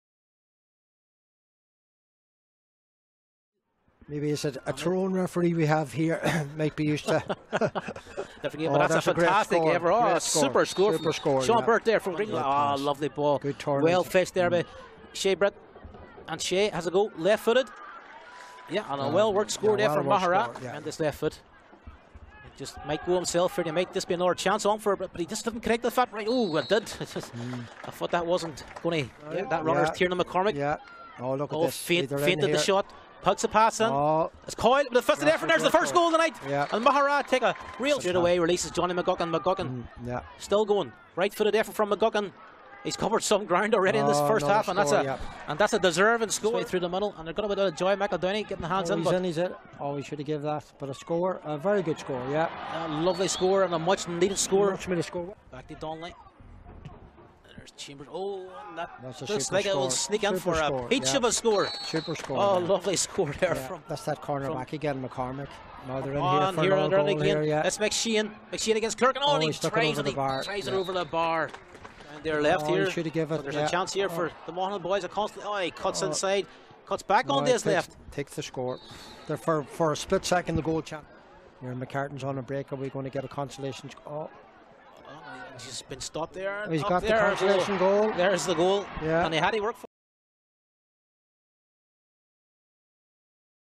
BeoSport Highlights: Rannafast Cup Final 2022
BeoSport Highlights of the Rannafast Cup Final 2022 between St. Patrick's Maghera and St. Macartan's Monaghan from the Dub Arena on Thursday 3rd March 2022.

St. Patrick's Maghera 1-15 St. Macartan's Monaghan 1-07